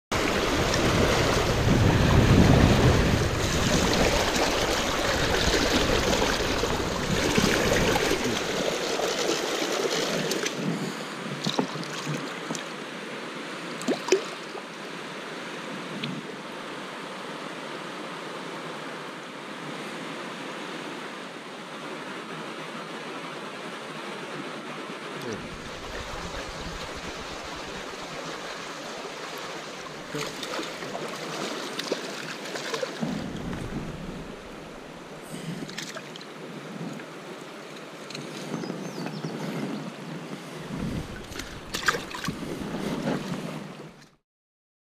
PA fly fishing
A quick look at a fishing adventure in Pennsylvania.  The Grannoms were hatching like I've never seen.  At times the air was full of them.